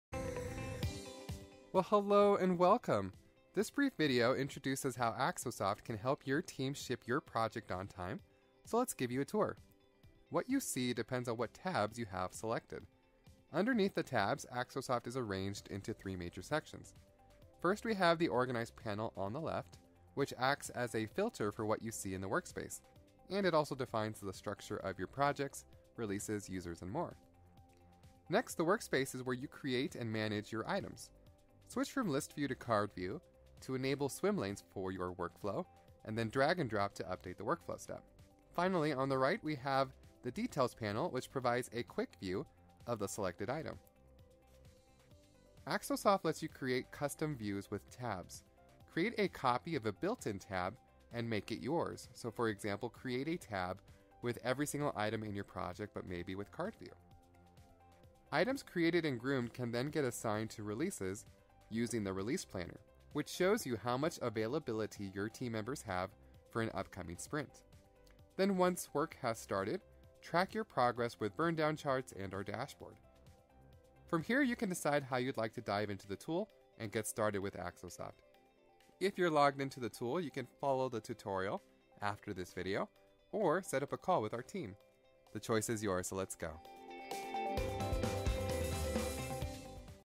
Axosoft: New User Intro
We bid your face welcome! This quick video will give you a quick tour of Axosoft's interface along with a high level summary of how to plan releases and keep track of progress.